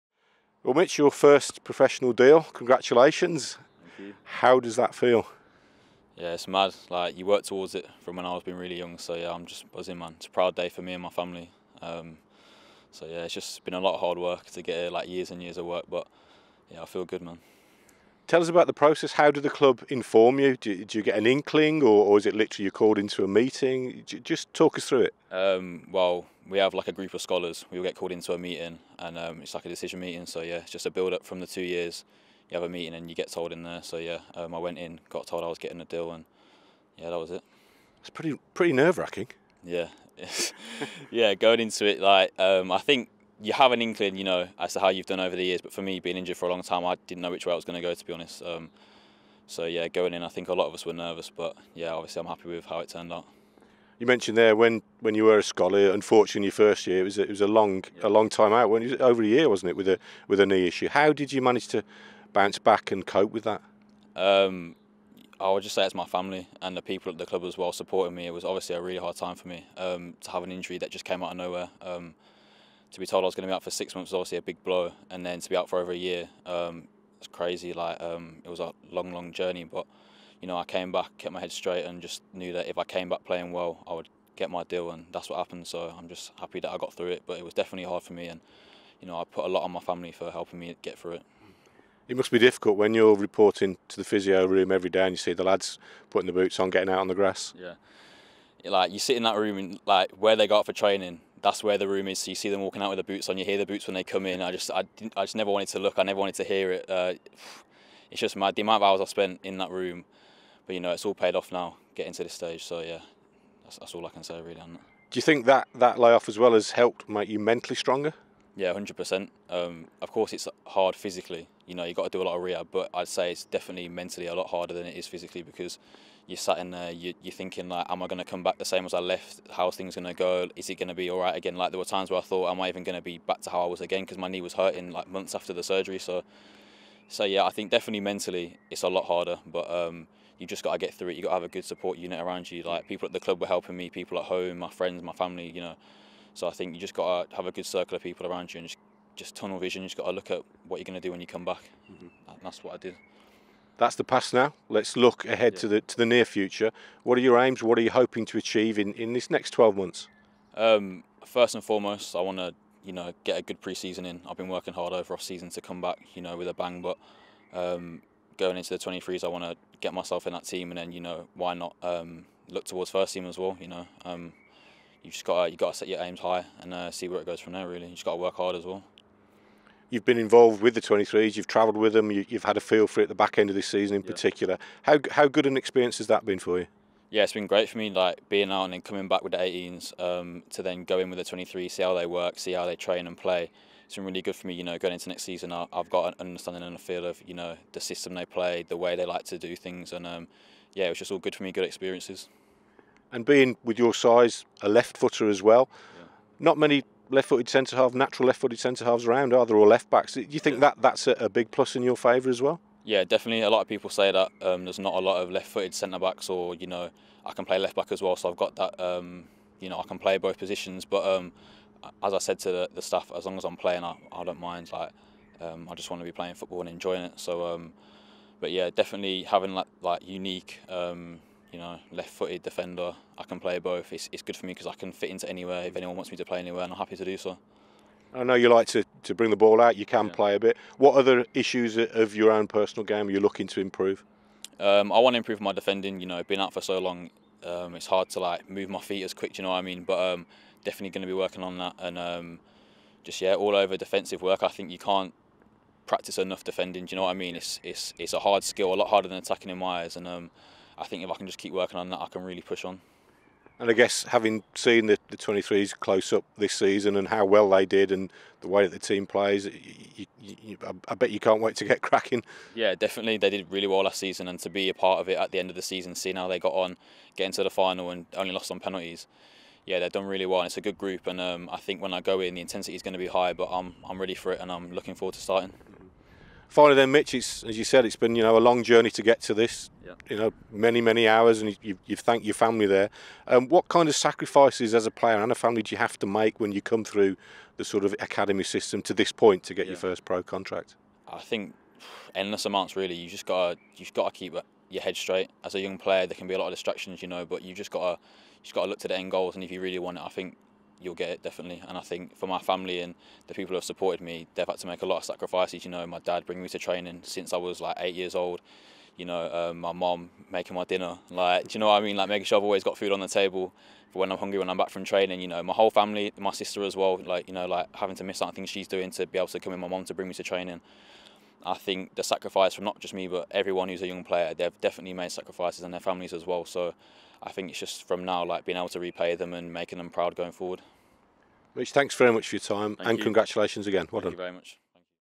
INTERVIEW | Mitch Roberts signs first pro deal
Centre-half Mitch Roberts has signed his first professional contract with the Club.